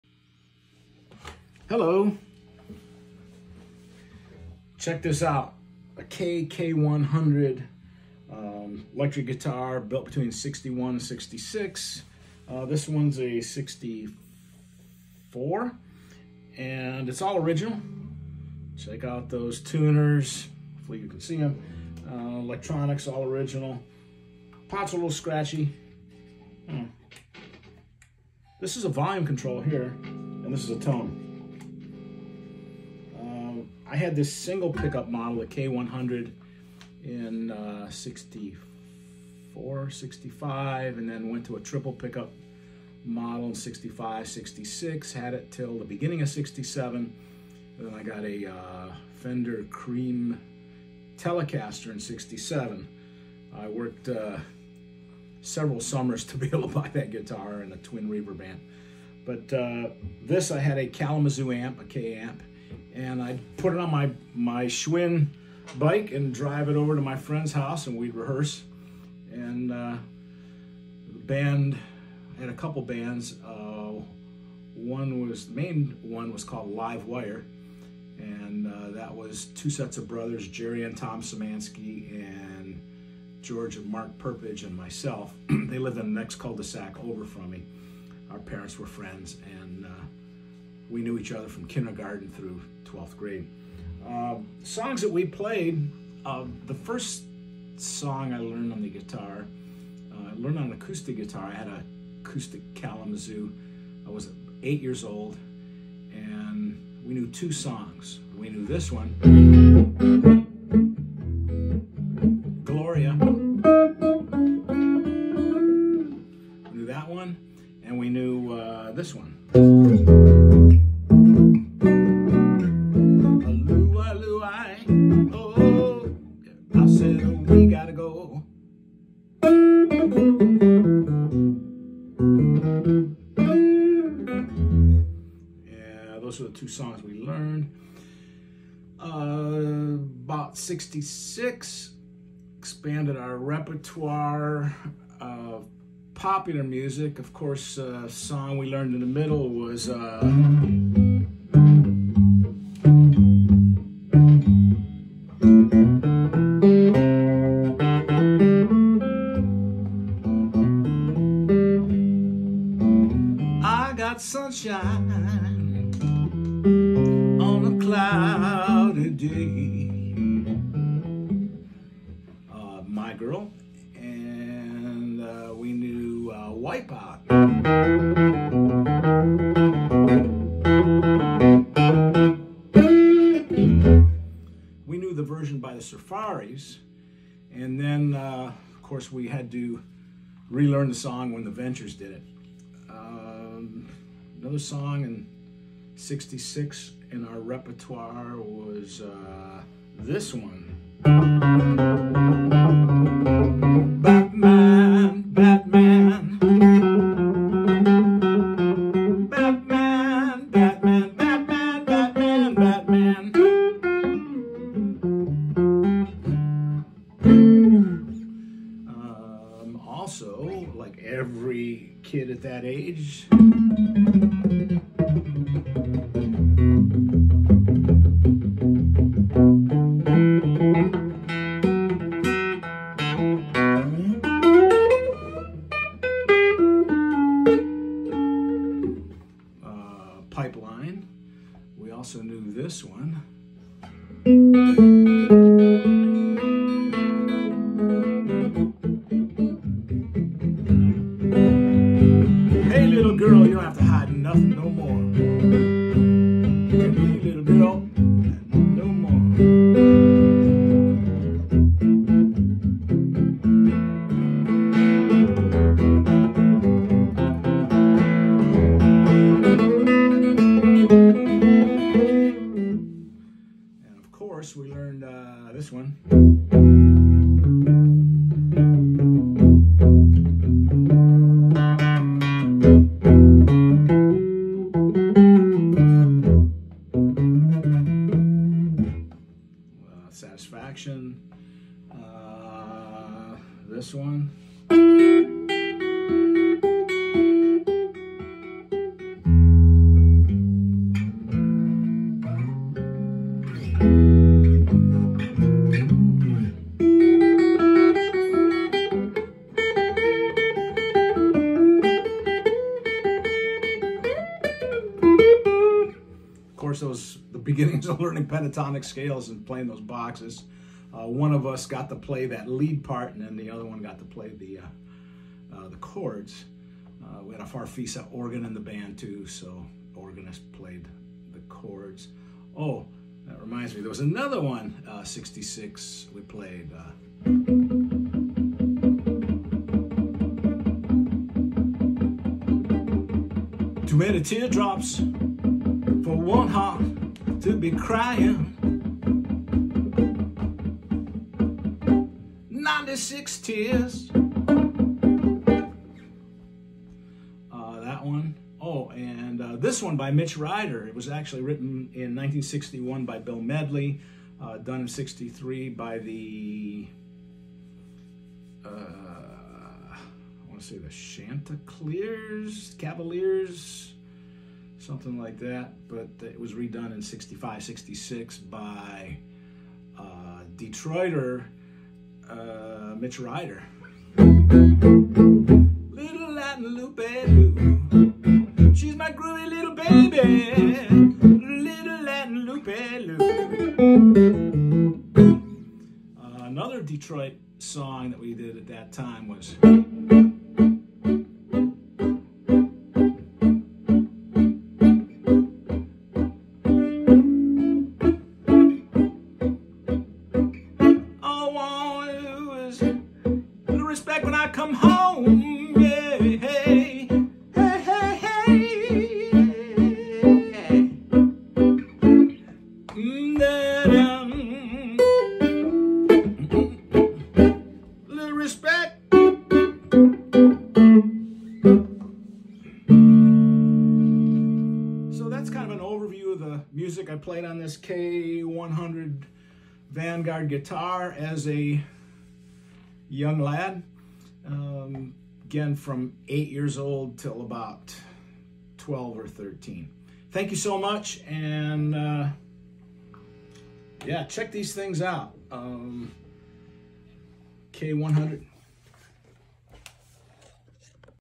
My First Electric Guitar
Kay Vanguard K100

My first electric guitar. Got the three pickup version shortly afterward.

Running through some of the basics period-correct repertoire.